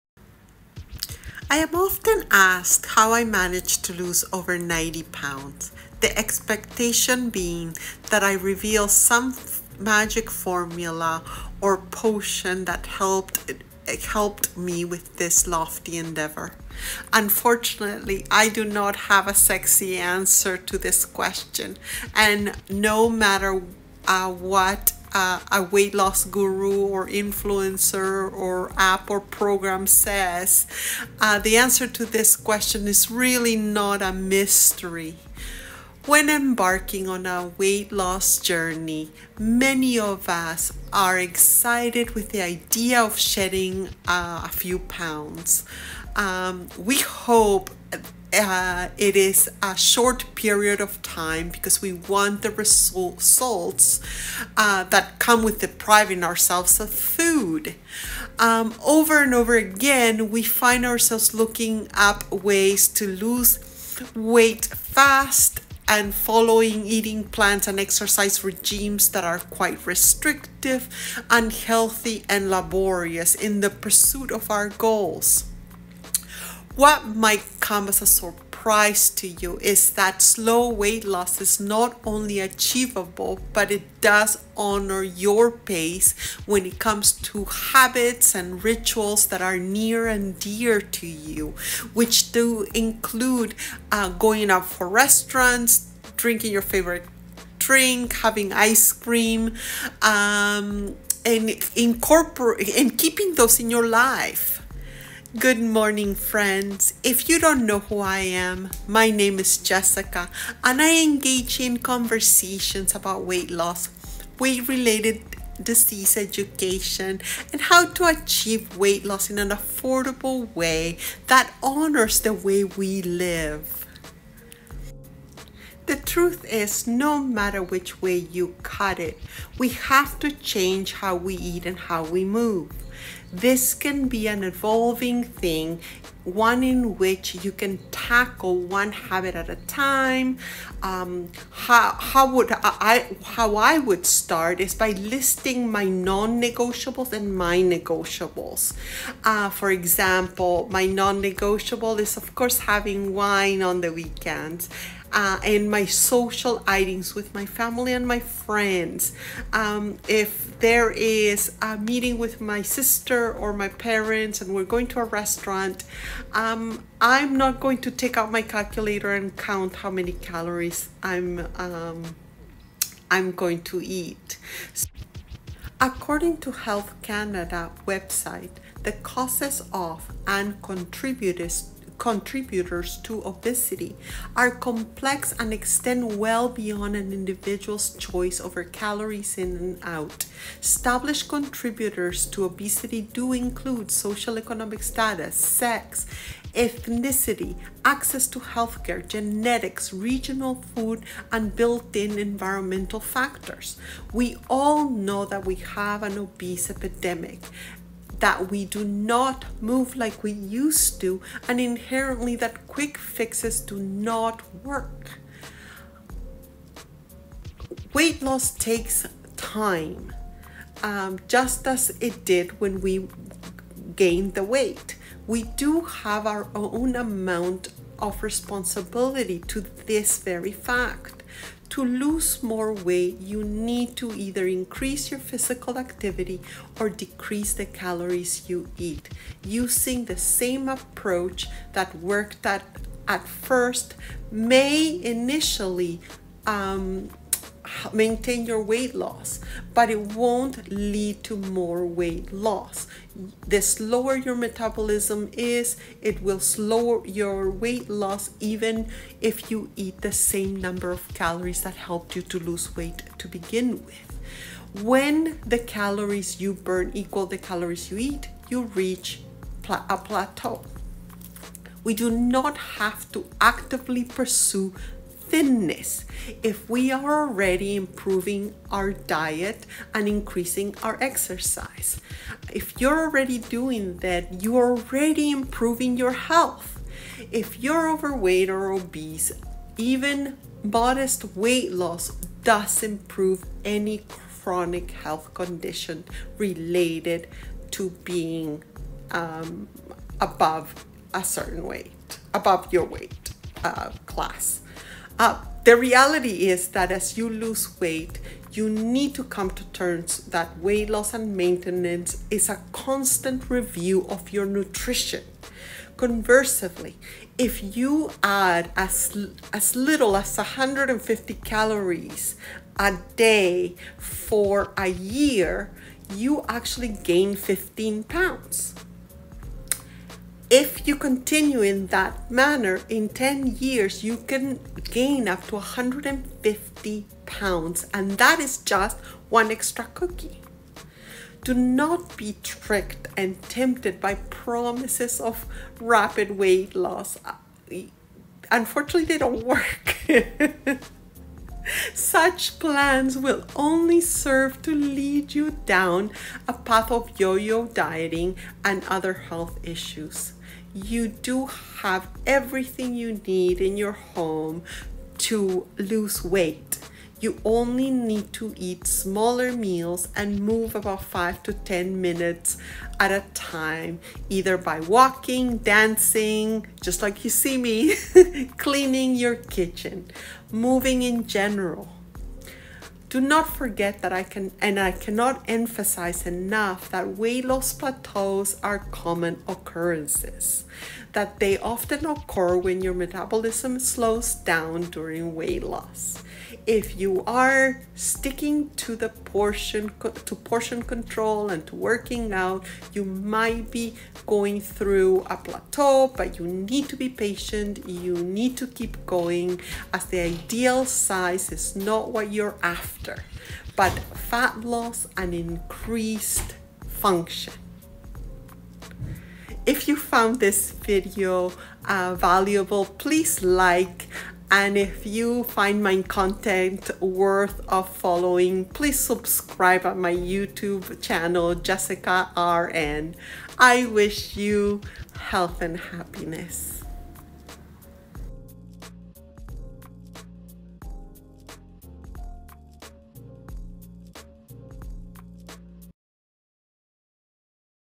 How I lost 90 pounds. Slow weightloss is best and why.
When embarking on a weight loss journey, many of us are excited with the idea of shedding pounds. We hope it is in a short period of time because we want to see the results of depriving ourselves of food. Over and over again, we find ourselves looking up ways to lose weight fast, following eating plans and exercise regimens that are quite restrictive, unhealthy and laborious in the pursuit of our goal. What might come as a surprise to you, is that slow weight loss is not only achievable but it does honour your pace when it comes to habits and rituals that are near and dear to you which includes going to restaurants and having your favourite foods continue to be in your life.